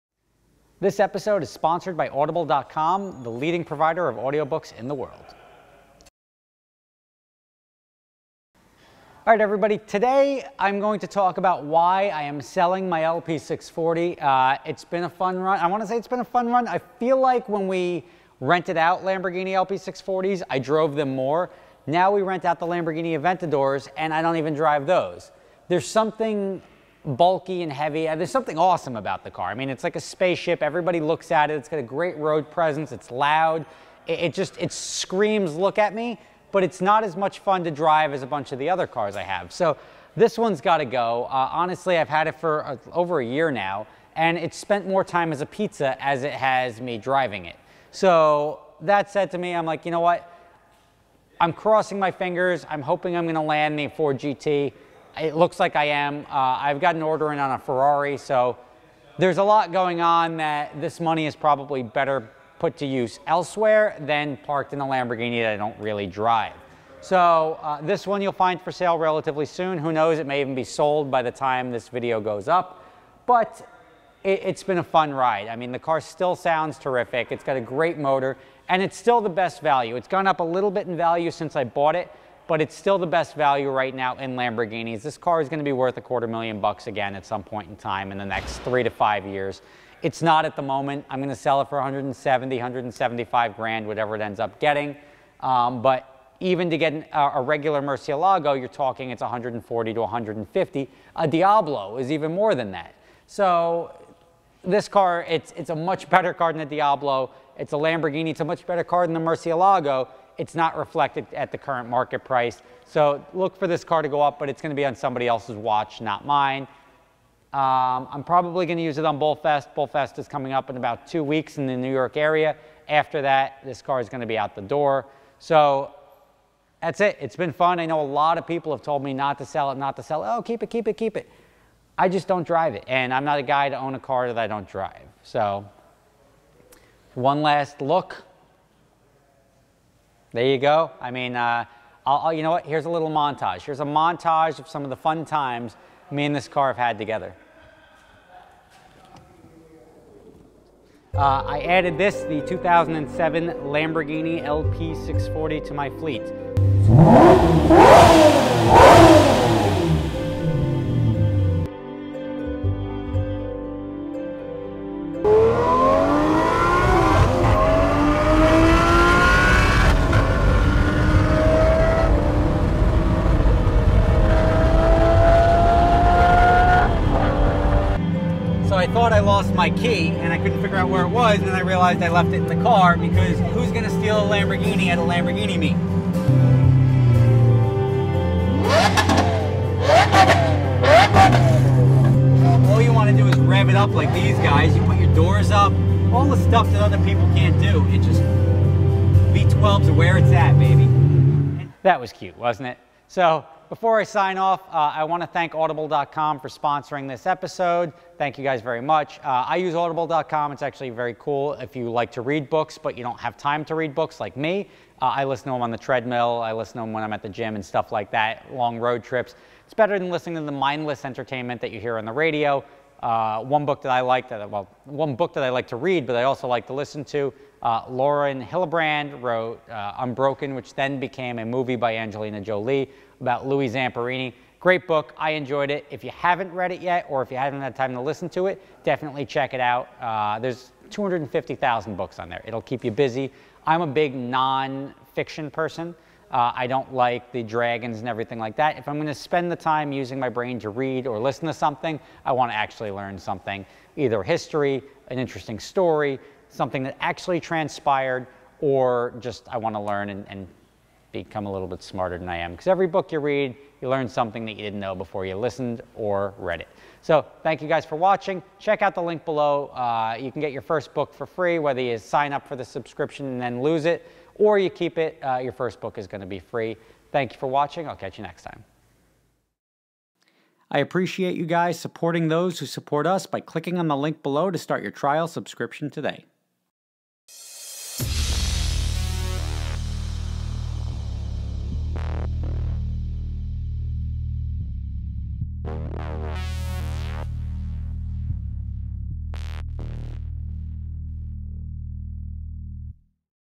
Why I am selling my Lamborghini
Its time to go, doesn't get driven nearly enough and the money can be used elsewhere.
for your free 30 day trial with access to over 250,000 audio books. Smart is sexy, books are good.

Best Cop Moments Part 8 due out next.